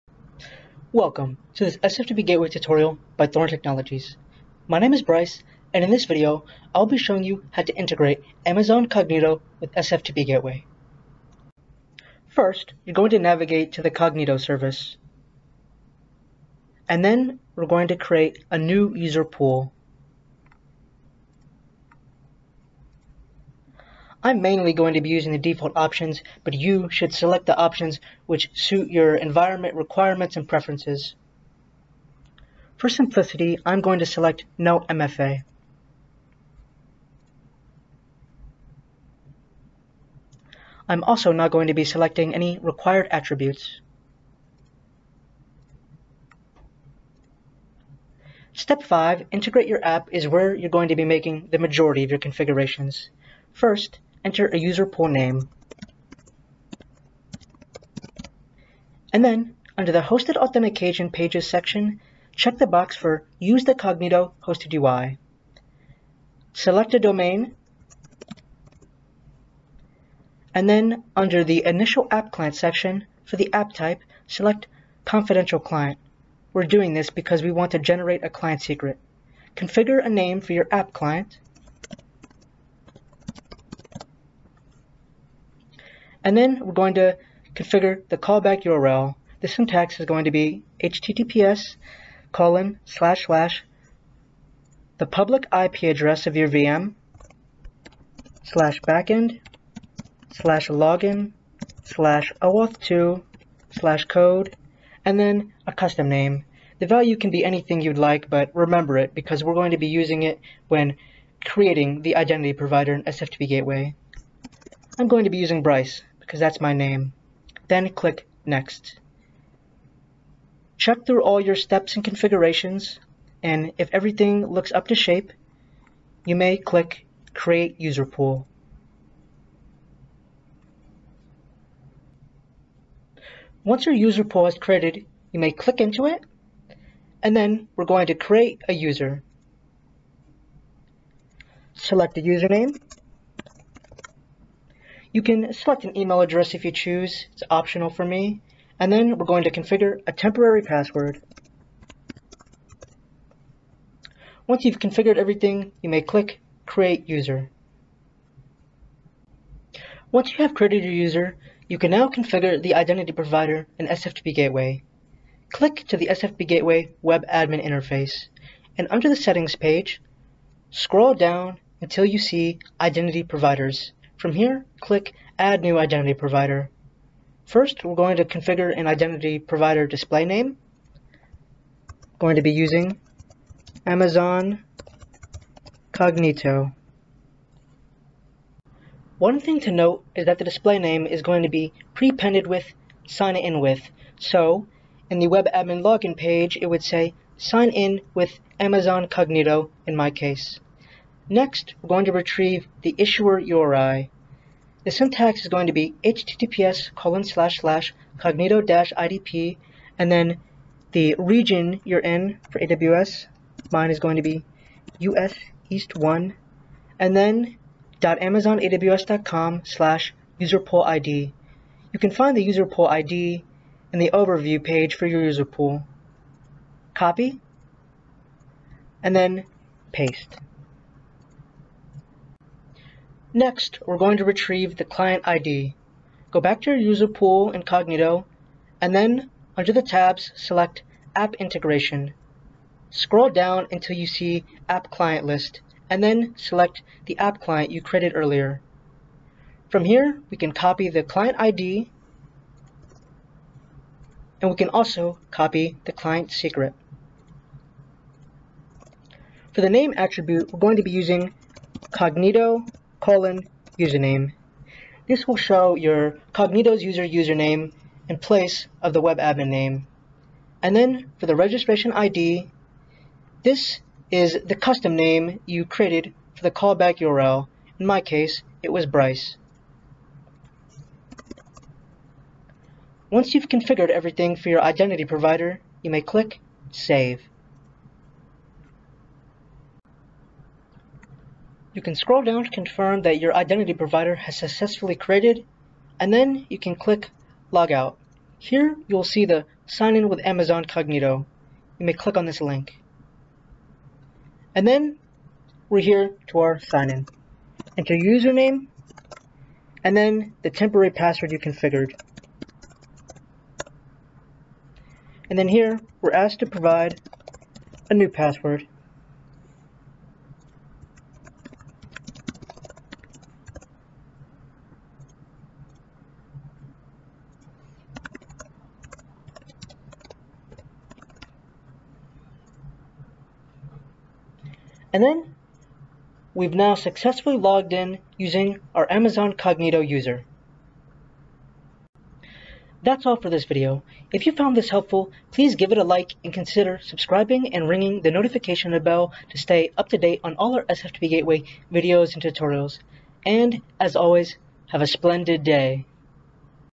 Integrating Amazon Cognito Authentication with SFTP Gateway for OAuth2 login
Get step-by-step instructions on how to integrate Amazon Cognito with SFTP Gateway Admin for OAuth2 login. These instructions allow you to manage administrator access to the SFTP Gateway Admin Dashboard from within Amazon Cognito.

For more on integrating Amazon Cognito with SFTP Gateway, check out the blog post

SFTP Gateway is a simple, secure and affordable way to give SFTP users access to cloud storage locations like Azure Blog Storage, Amazon S3 and Google Cloud Storage.